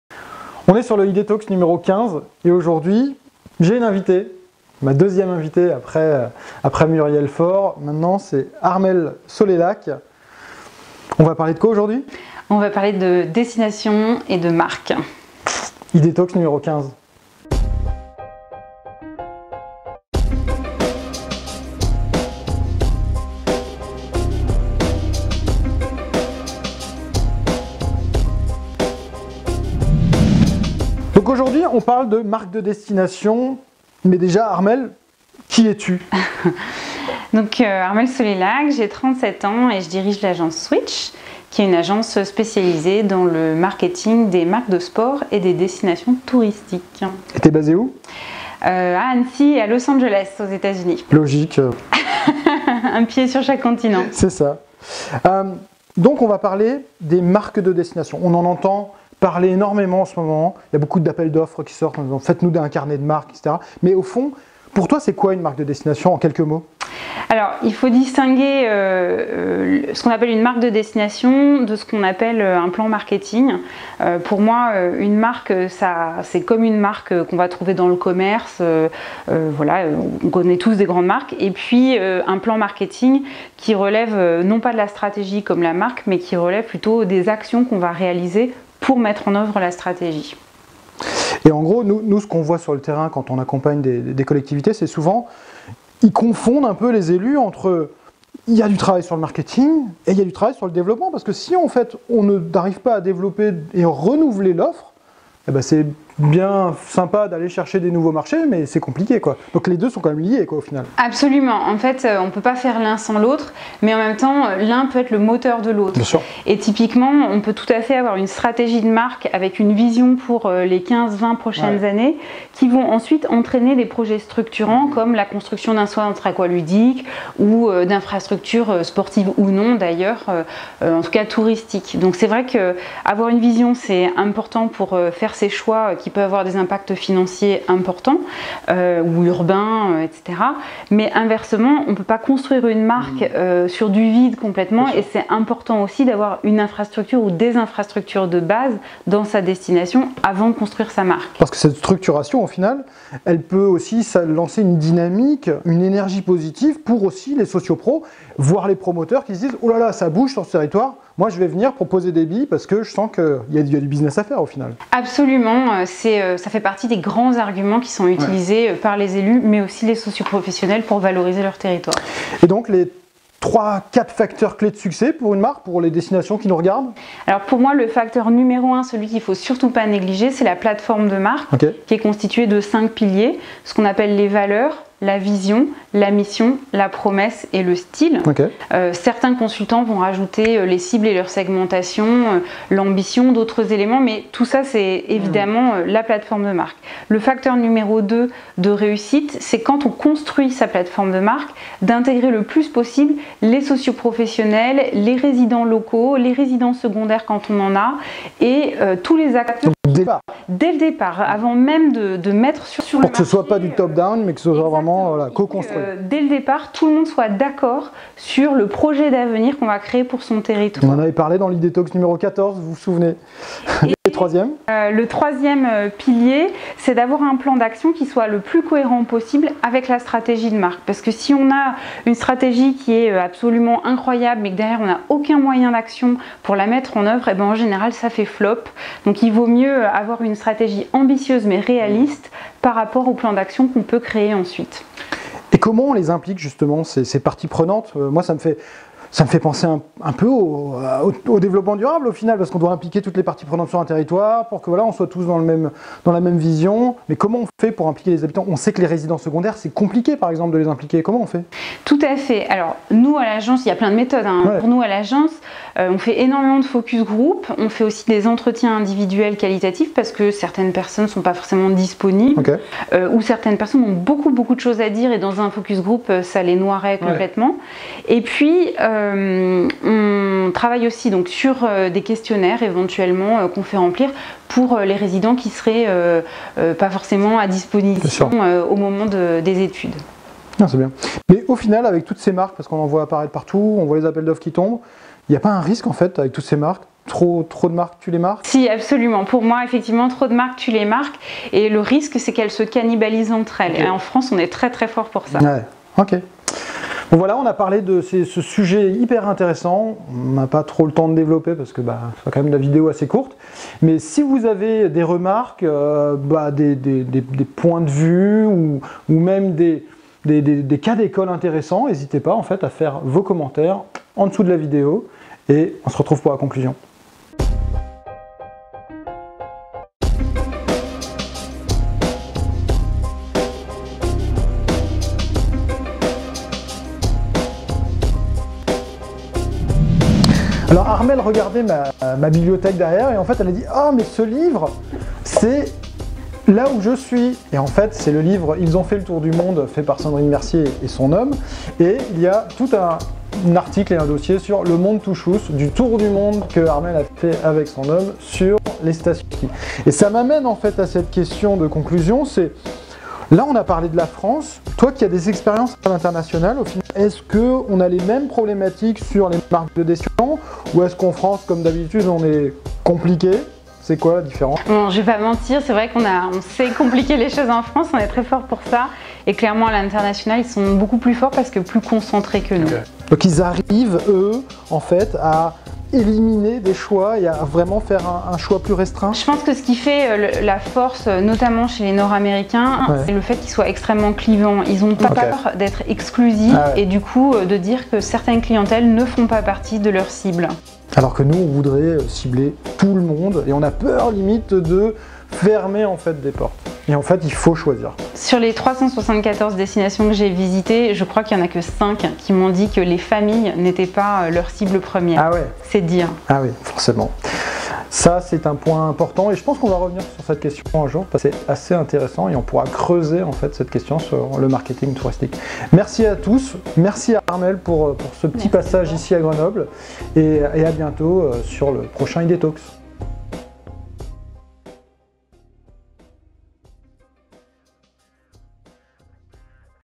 ID TALKS - EPISODE 15 - Rencontre avec Armelle Solelhac, CEO de SWiTCH
Elles veulent toutes leur marque! Les destinations se transforment! Chacun veut son logo, sa tagline, son carnet de valeurs et son portrait identitaire. Est-ce que ce nouvel engouement va dans le bon sens? A quel moment faut-il impliquer les parties prenantes du territoire pour en faire une marque vraiment partagée? Armelle Solelhac, CEO de SWiTCH fait le point avec nous sur ce nouvel épisode en duo d'ID-TALKS!